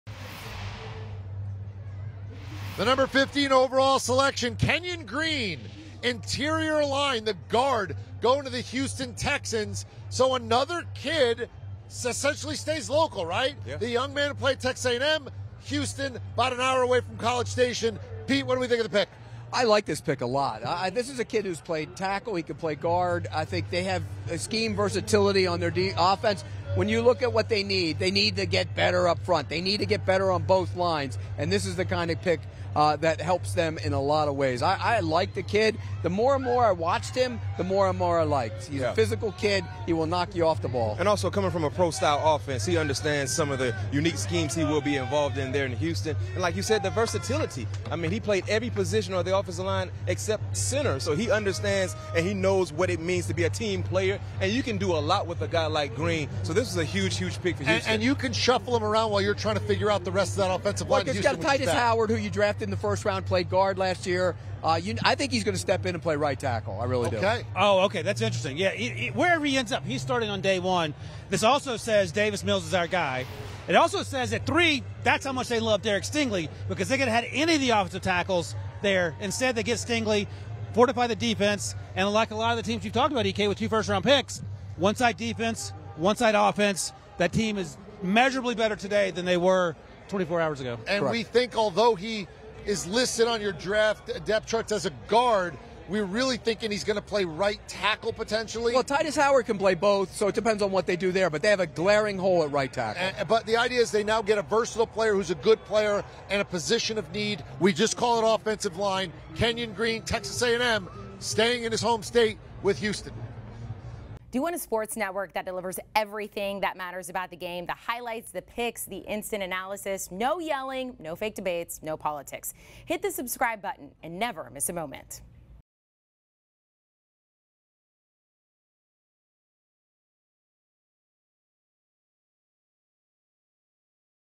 Texans take PHYSICAL OL Kenyon Green With No. 15 Pick I 2022 NFL Draft Grades
Pete Prisco joins CBS Sports HQ to grade the Texans selecting Kenyon Green with the No. 15 pick in the 2022 NFL Draft.